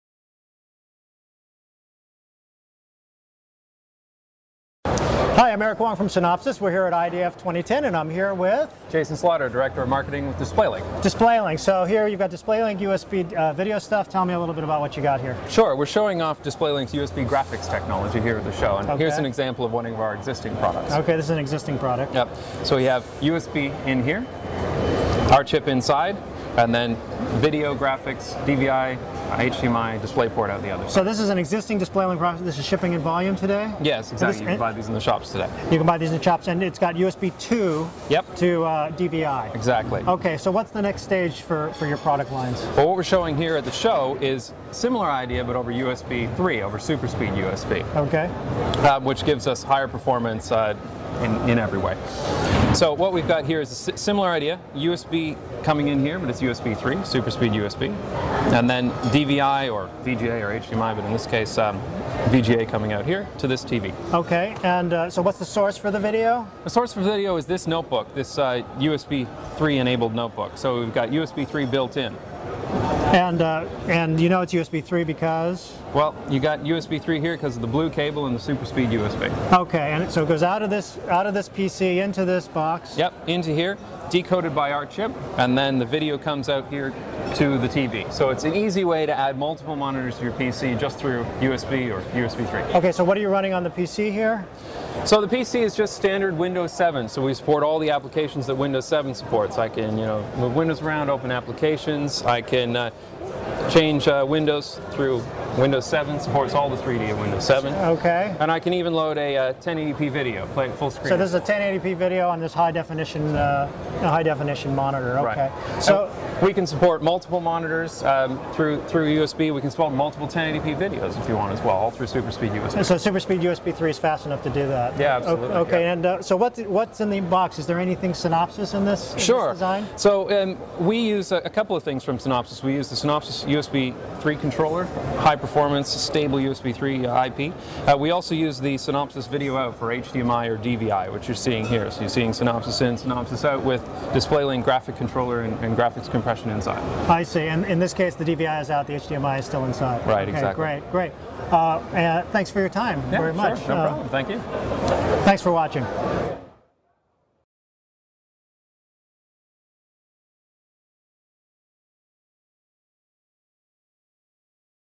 DisplayLink demonstrates HD Video streaming 1080p over USB 3.0 | Synopsys
DisplayLink demonstrates HD Video streaming 1080p over USB 3.0